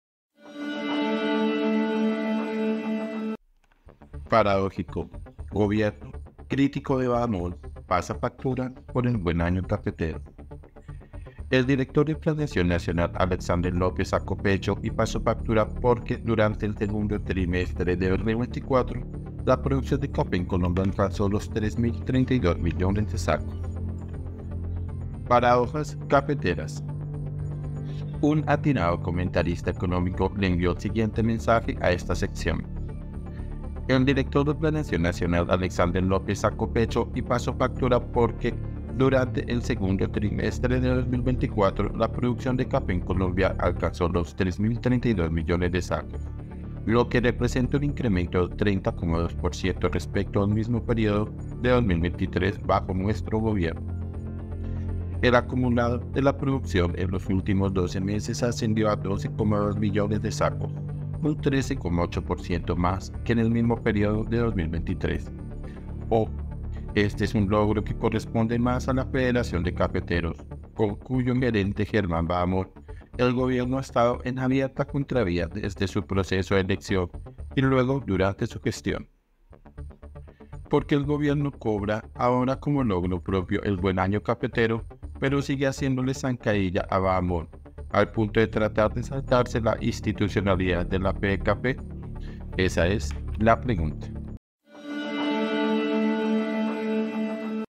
PARADÓJICO 🟥 GOBIERNO, CRÍTICO DE BAHAMÓN, "PASA FACTURA" POR EL BUEN AÑO CAFETERO
Un atinado comentarista económico le envió el siguiente mensaje a esta sección: “El director de Planeación Nacional, Alexánder López, sacó pecho y pasó factura porque ‘durante el segundo trimestre de 2024, la producción de café en Colombia alcanzó los 3,032 millones de sacos, lo que representa un incremento del 30,2 % respecto al mismo periodo de 2023 bajo nuestro gobierno.